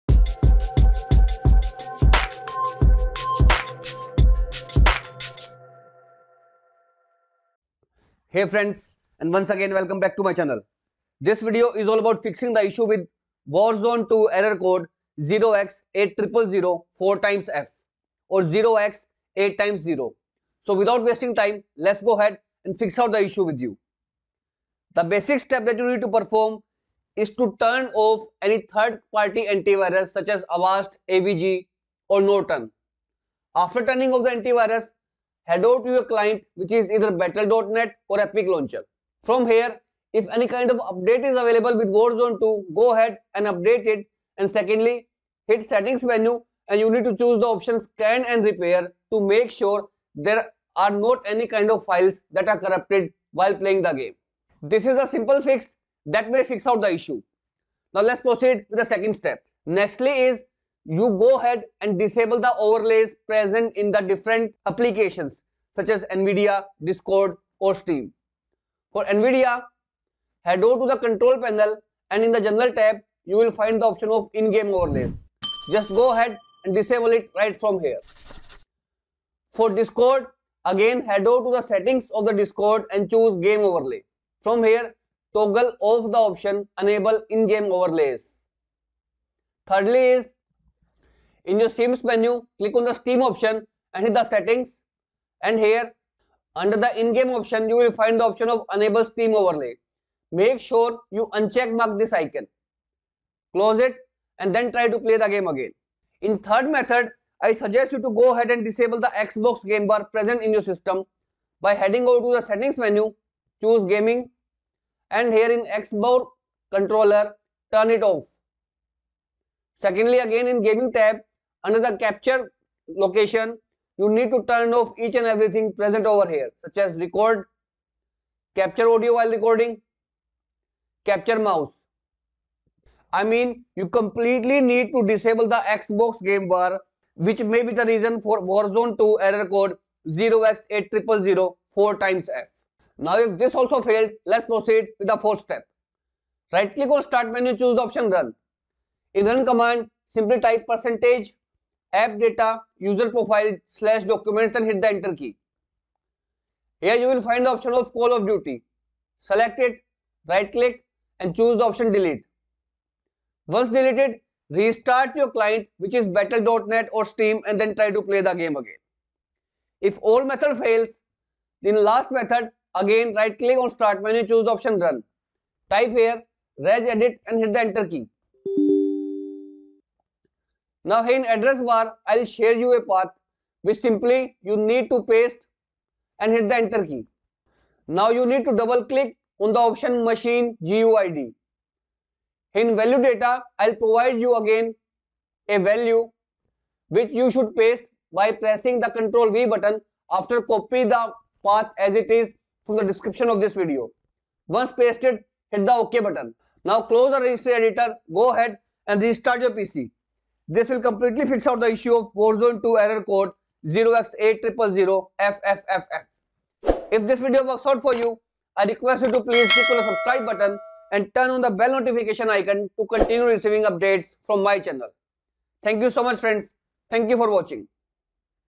MW2 Warzone 2 Game_Steam_ship.exe Crashing Error code x8000FFFF(X00000000)
Modern warfare 2 is the latest newest release of call of duty but it has crashing error such as game_steam_ship.exe and then the game crashes. This crash causing the game unplayable at all. so this video will help you to fix the warzone 2 error game steam ship.exe crash

00:00 Introduction
00:24 Method 1 by updating the game and disabling antivirus
00:56 Method 2 by disabling ovverlays of discord, nvidia
01:47 Method 3 by disabling the xbox gamebar
02:24 Method 4 by deleting the saved data
02:52 Method 5 using the registry editor

path to paste in run command :
%UserProfile%\Documents\

path to paste in regedit :
Computer\HKEY_LOCAL_MACHINE\SOFTWARE\Microsoft\Cryptography

value to paste in field :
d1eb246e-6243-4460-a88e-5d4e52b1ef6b